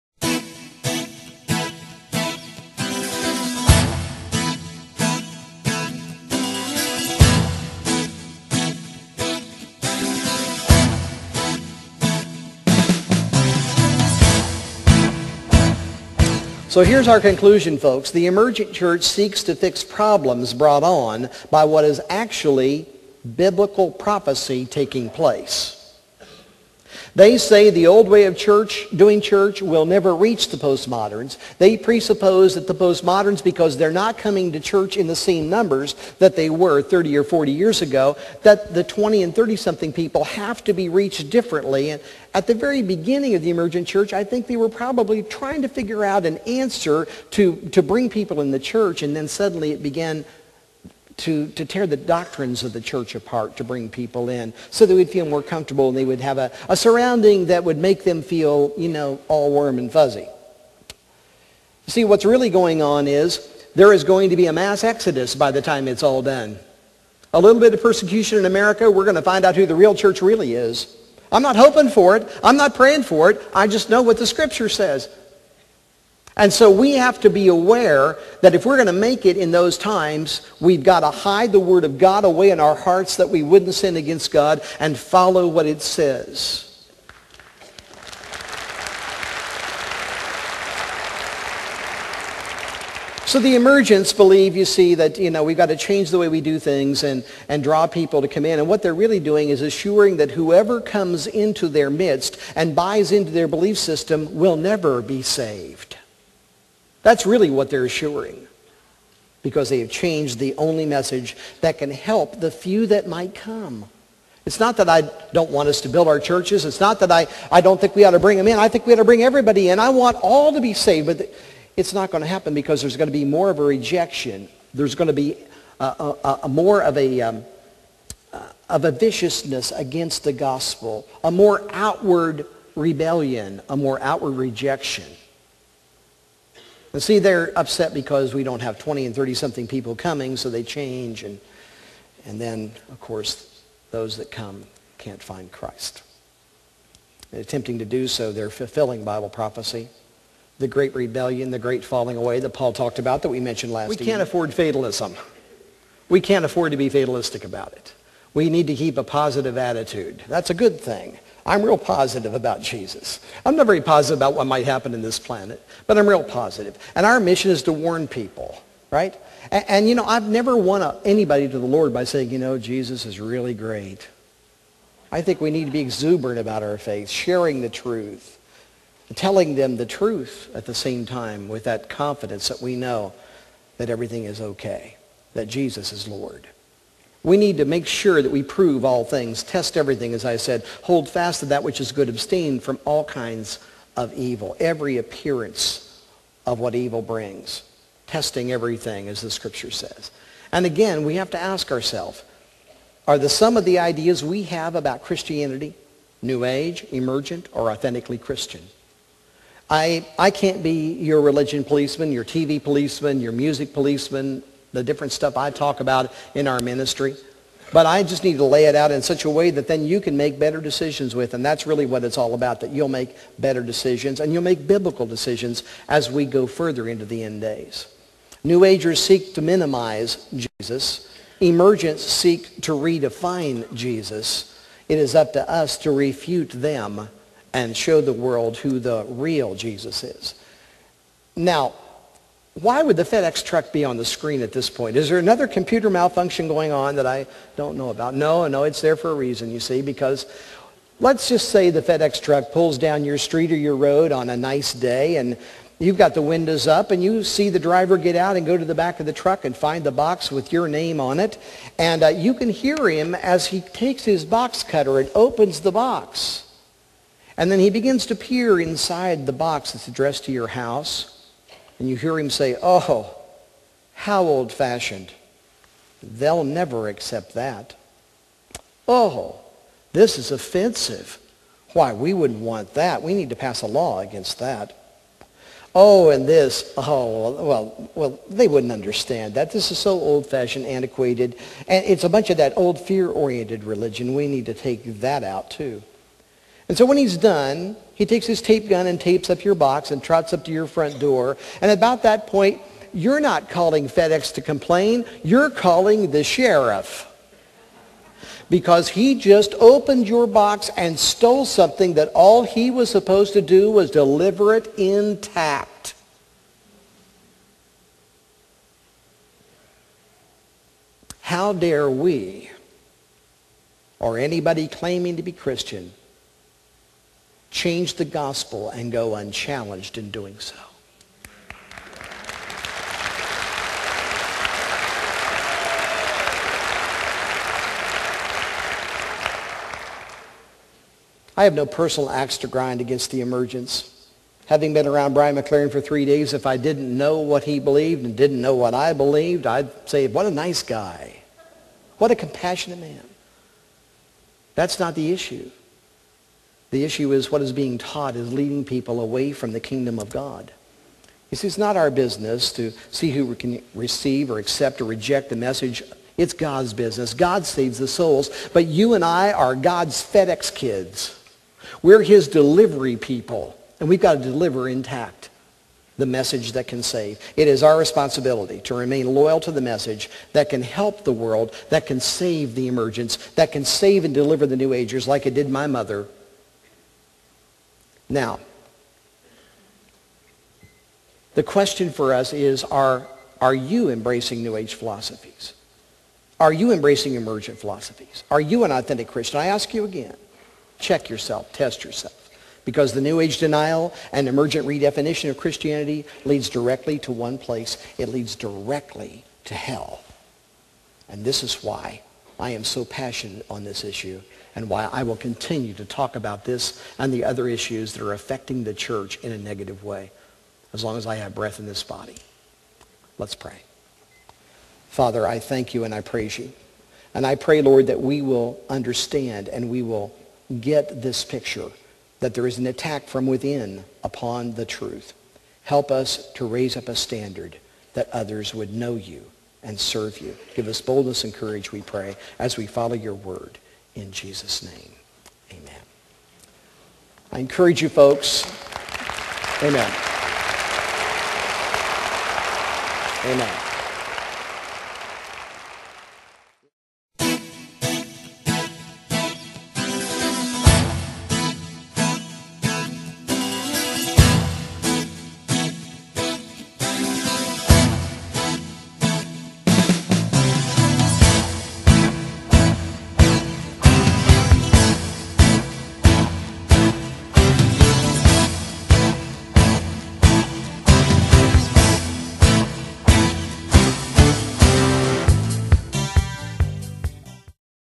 The Importance of Keeping the Gospel Intact with Eric Barger
Recorded live at Jan Markell's "Understanding the Times 2009" Conference. Eric Barger emphasizes the importance of keeping the Gospel intact as he concludes the message titled "Is Your Church Emergent, New Age or Christian?" (Oct 10, 2009)